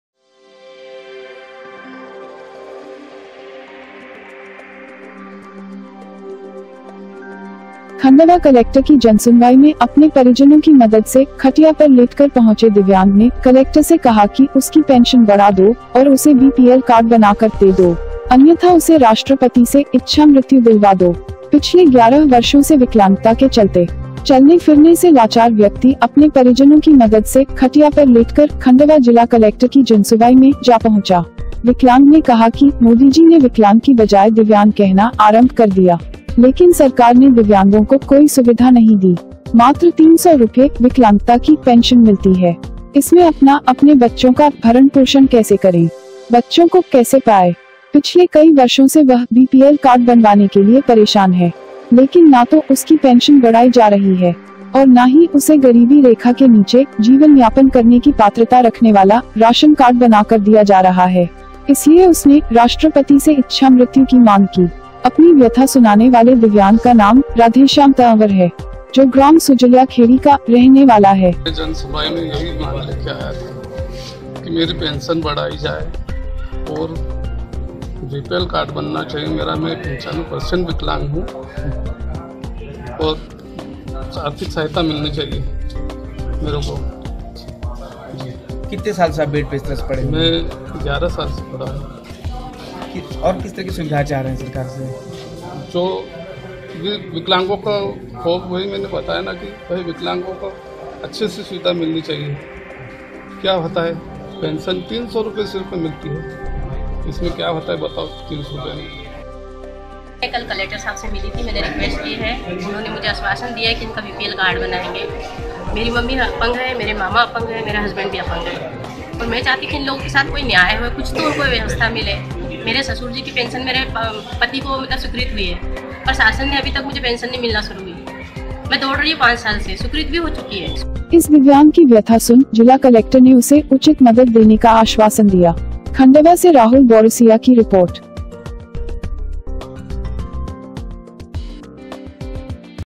खटिया पर लेटकर पहुंचे दिव्यांग ने कलेक्टर को बताई अपनी पीड़ा - MP NEWS NETWORK KHANDWA
खण्डवा कलेक्टर की जनसुनवाई में अपने परिजनों की मदद से खटिया पर लेटकर पहुंचे दिव्यांग ने कलेक्टर से कहा कि उसकी पेंशन बढ़ा दो और उसे बी पीएल कार्ड बनाकर दे दो। अन्यथा उसे राष्ट्रपति से इच्छा मृत्यु दिलवा दो